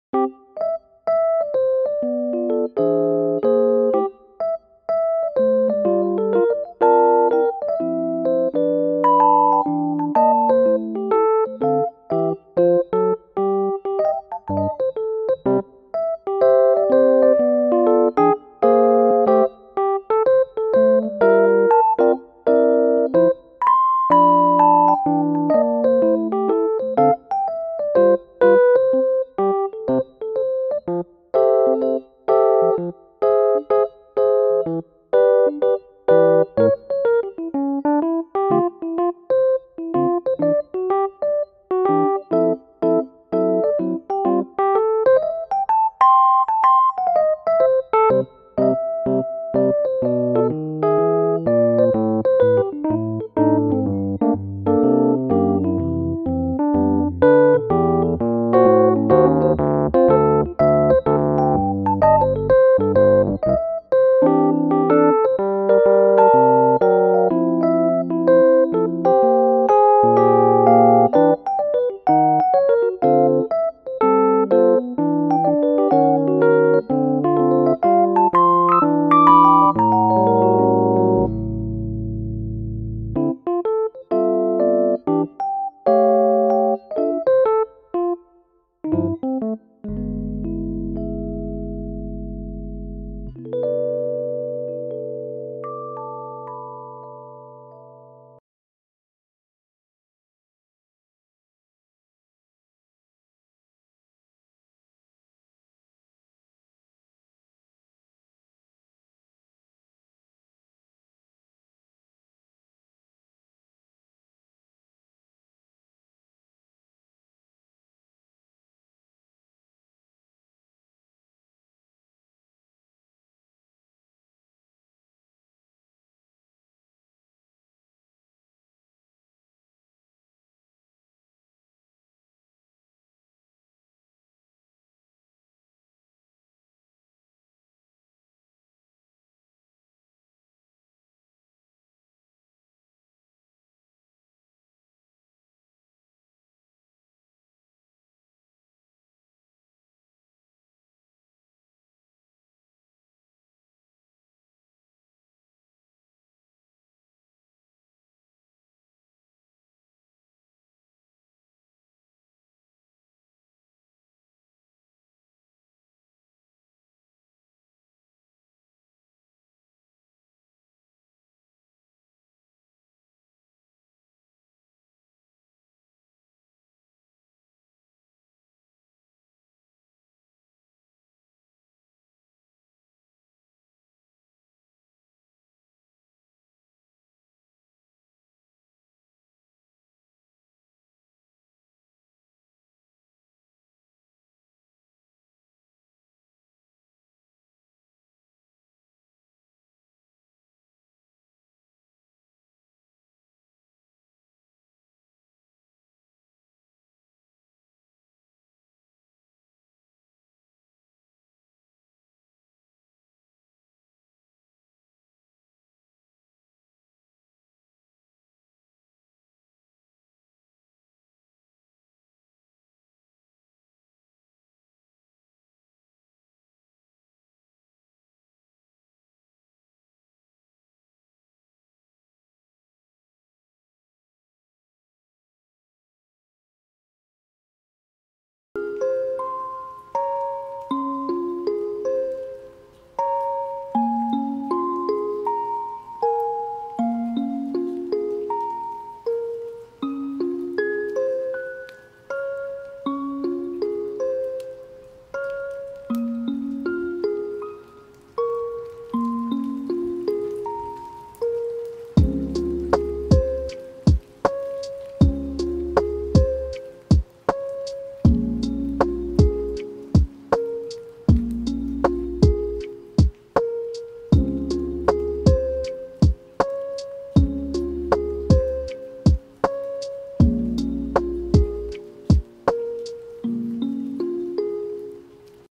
drawing your OCs in my style ✦ procreate speedpaint @webbidee persona ♡
hii! please keep commenting under my last post to mention your art account where I can see your ocs and draw them ♡
( remember to keep your account public )

✩ equip✩
procreate
brushes : anime pack on my gumroad